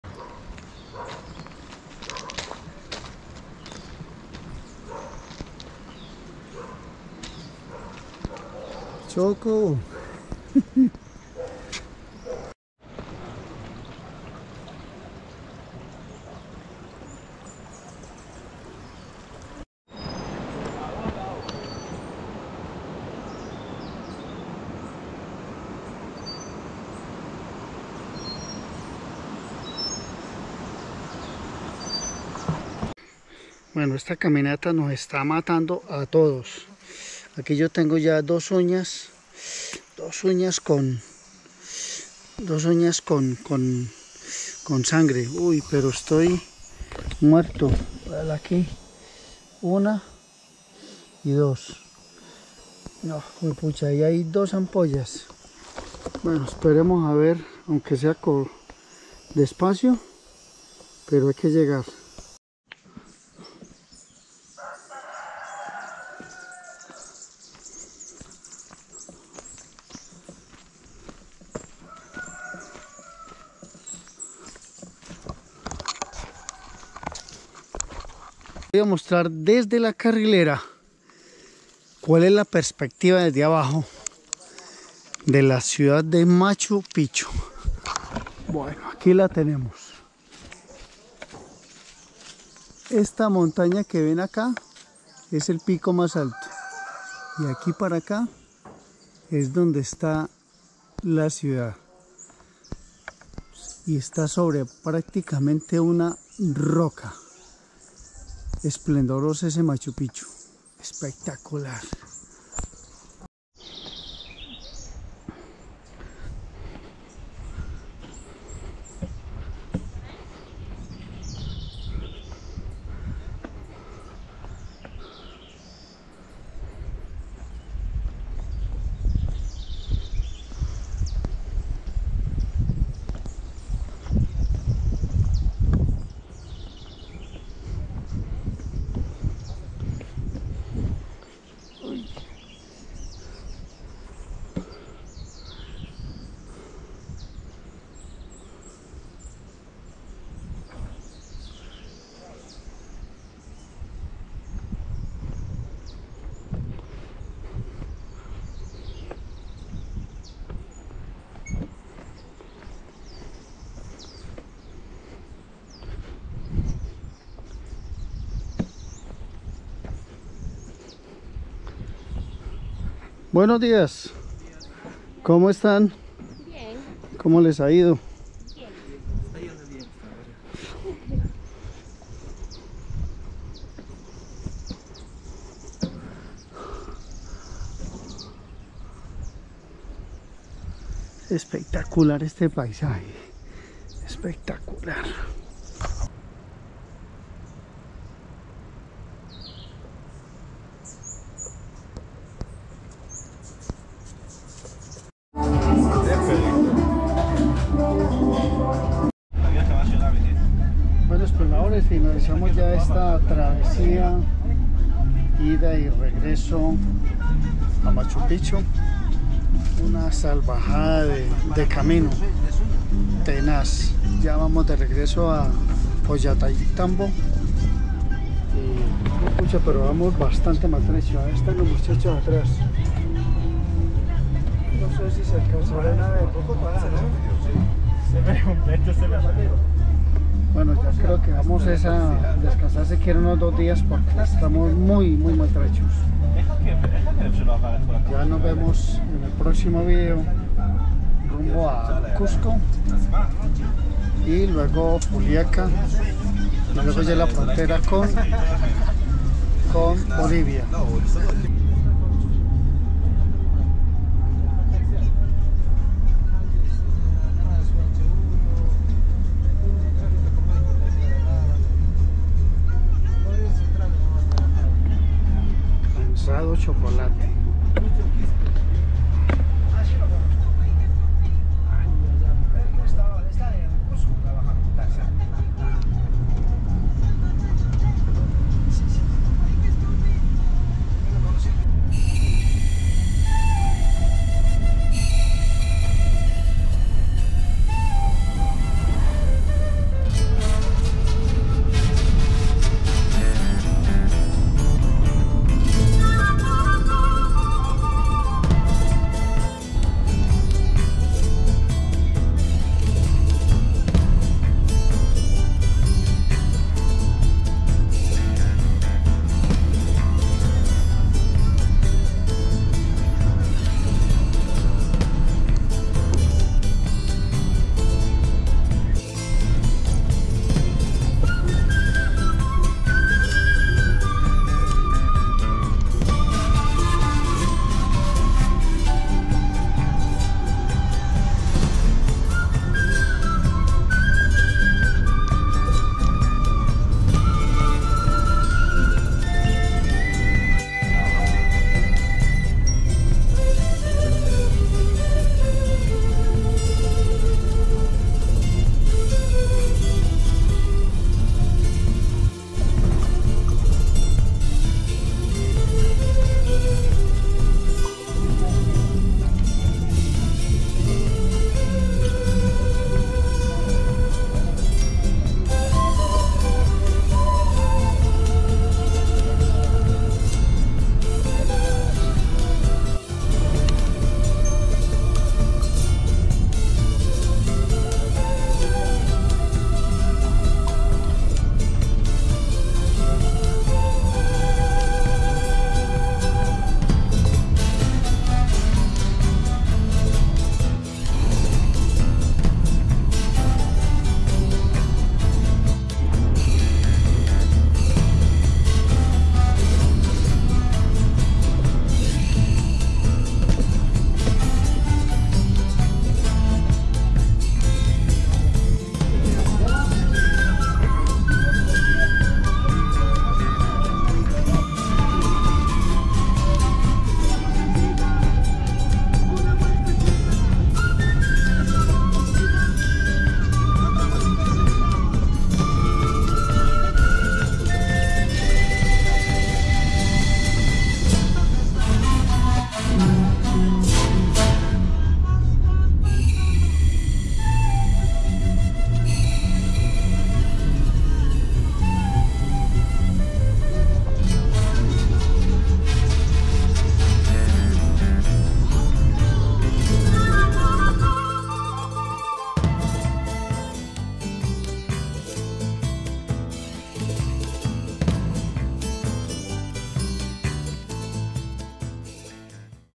LA RUTA IDEAL PARA IR Y VOLVER A MACHUPICHU, POR LA HIDROELECTRICA, , PERU, julio 2024
CUANDO REALIAZAMOS EL REGRESO A LA LOCALIDAD DE OLLANTAYTAMBO, NOS PERCATAMOS DEL ERROR TAN GARRAFAL, QUE HABIAMOS COMETIDO POR UNA MALA INFORMACION DE UN SEÑOR, QUE NOS DIO UNA MALA INFORMACION, PERO BUENO, ESTAMOS DE AVENTURA, Y EN SU MOMENTO SE RECORDARA. COMO UNA. ANECDOTA DE TERAVESIA .
YOUTUBE. CHOCOEXPLORADOR
FACEBOOK. MOTEROSBICIEXPLORADORES
INSTAGRAN. MOTEROSBICIEXPLORADORES
TIKTOK. MOTEROSBICIEXPLORADORES